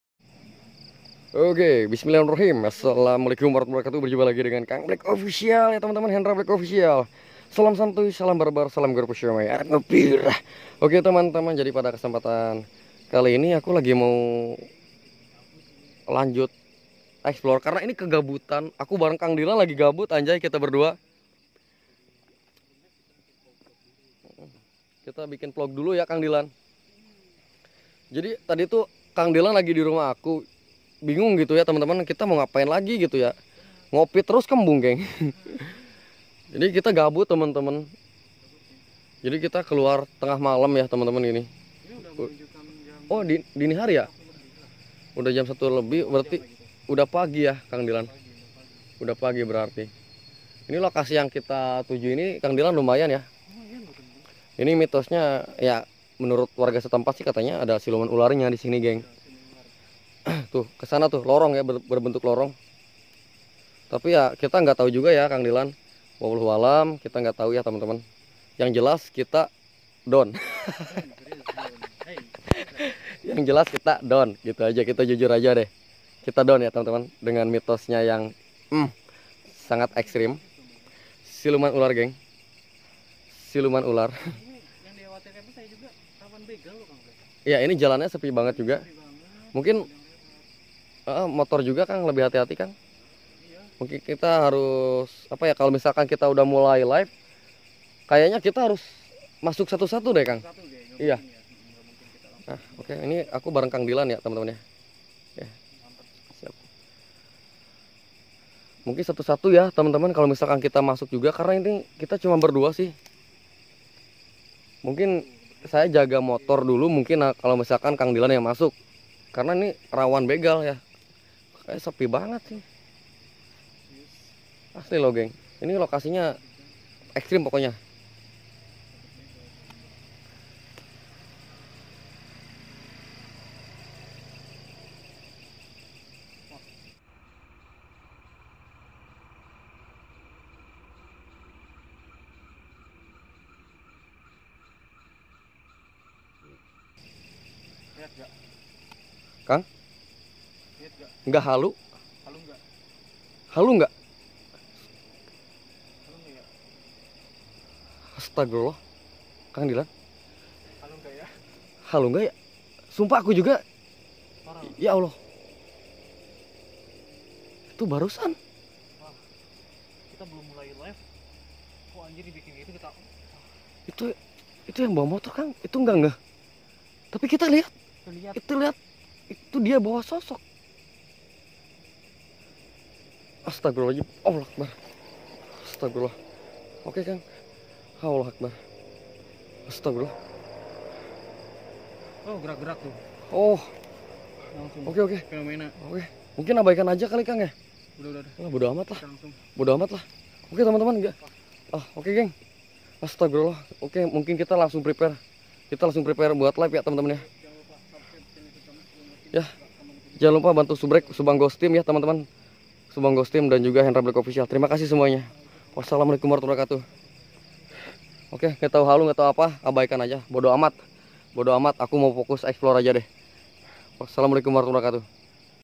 PENGENDARA MOTOR GAK SADAR BONCENG KUNTILANAK !!
Assalamualaikum.. pada kesempatan kali ini saya mendapatkan sebuah momen yang tidak di sengaja yaitu adanya pengendara motor yang membawa sosok kuntilanak ketika saya akan melakukan kegiatan explore, buat yang penasaran yuk simak terus video nya gengs!